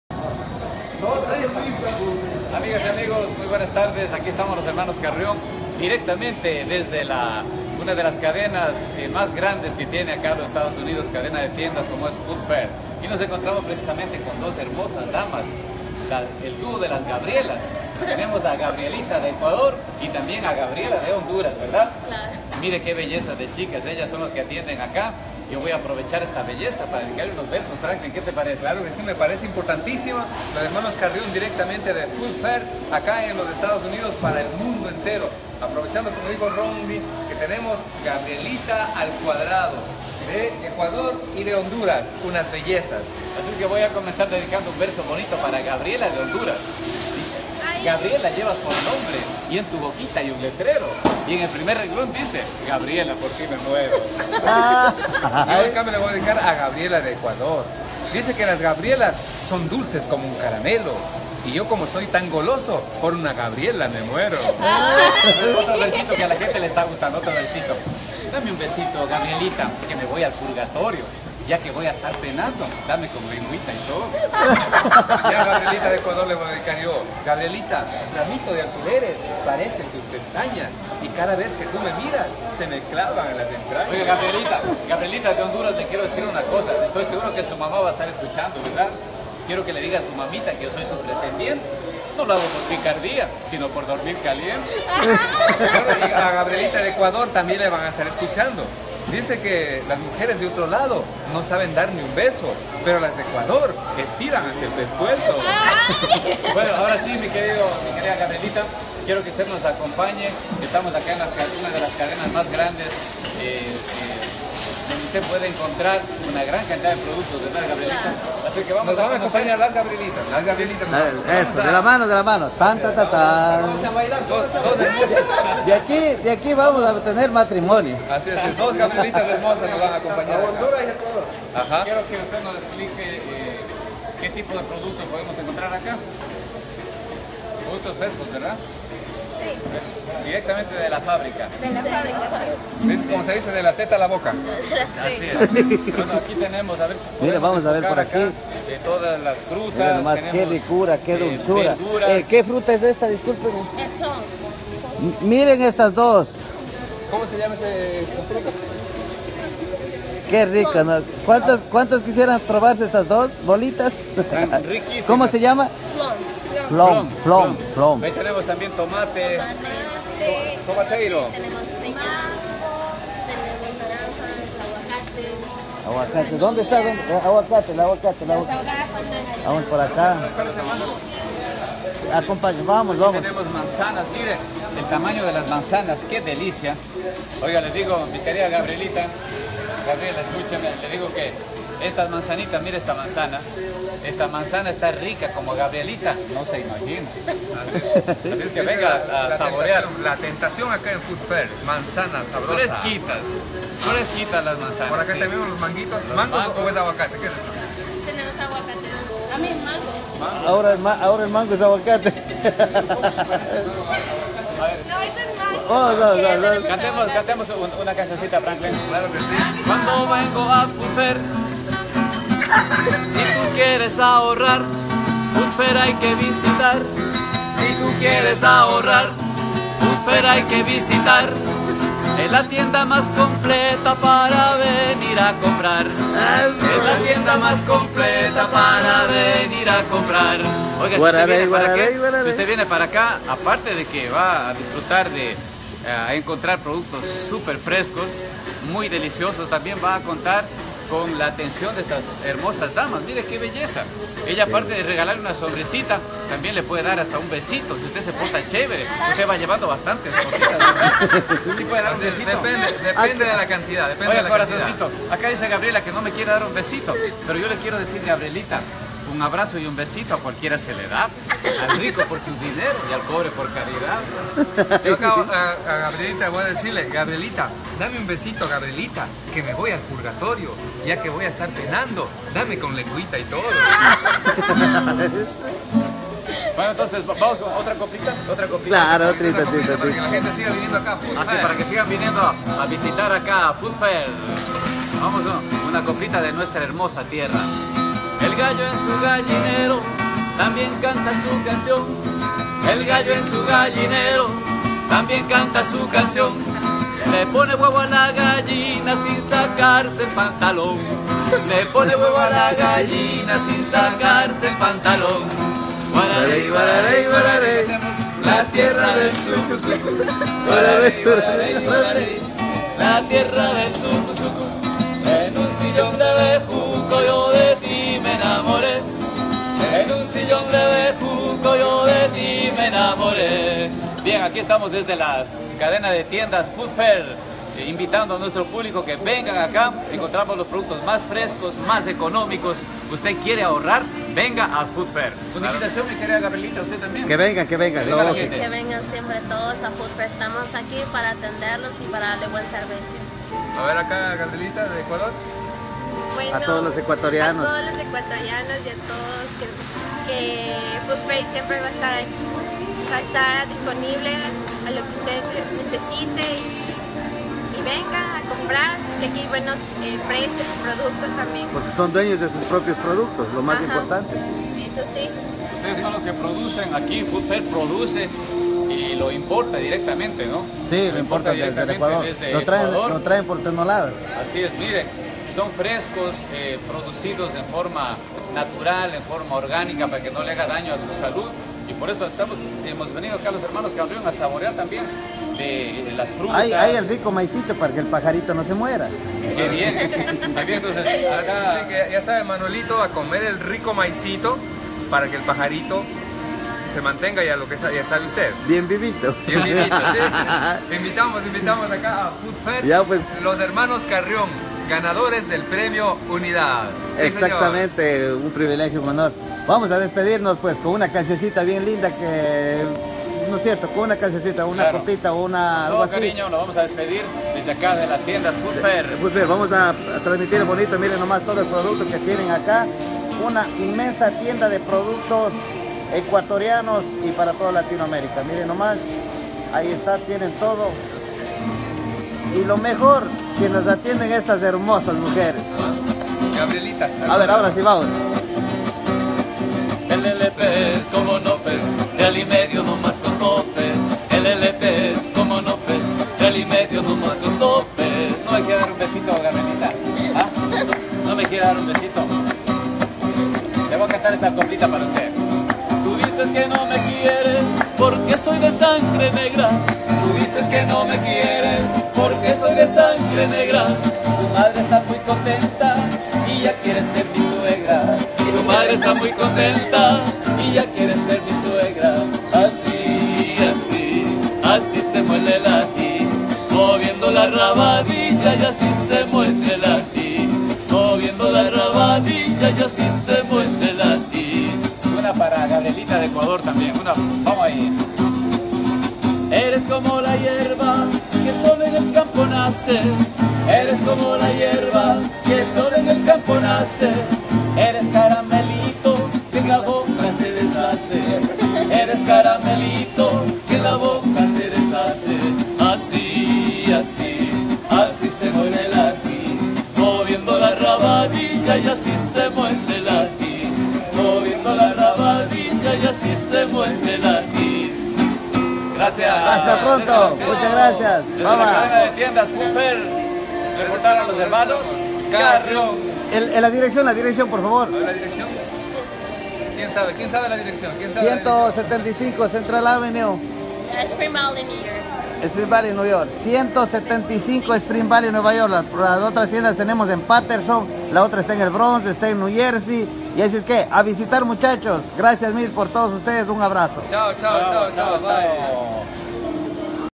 Como enamorar con coplas /Hermanos Carrion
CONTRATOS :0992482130

Las coplas más lindas para enamorar a hermosas damas de Ecuador y Honduras   con Los
Hermanos Carrion desde New York